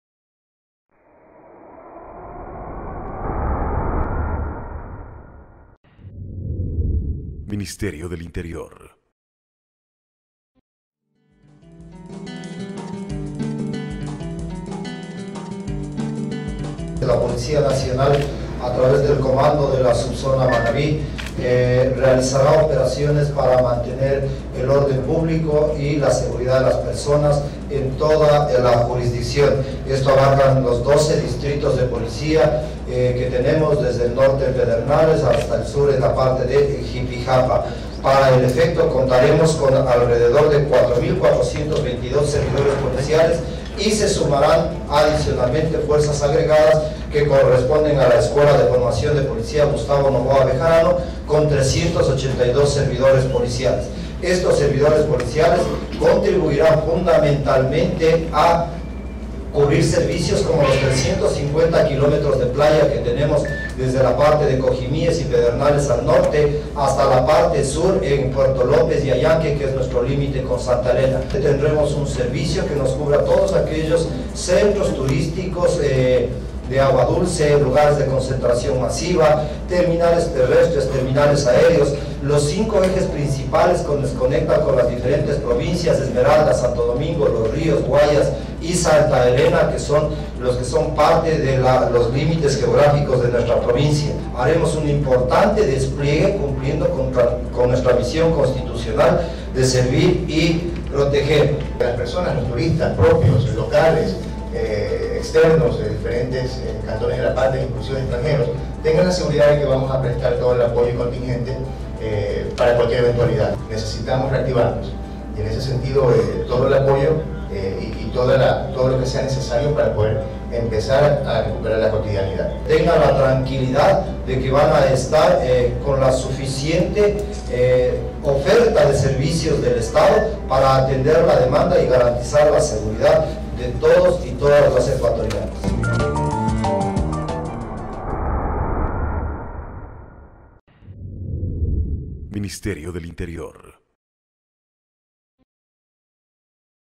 4.812 policías precautelan la seguridad de las personas durante el feriado, en Manabí
Portoviejo. La Policía Nacional puso en ejecución el operativo de seguridad, antes, durante y después del feriado del 24 de mayo. El dispositivo que tiene como finalidad resguardar la integridad de las personas y sus bienes, entró en vigencia a las 12h00 de este jueves 26 de mayo y finalizará el lunes 30 de mayo, a las 06h00.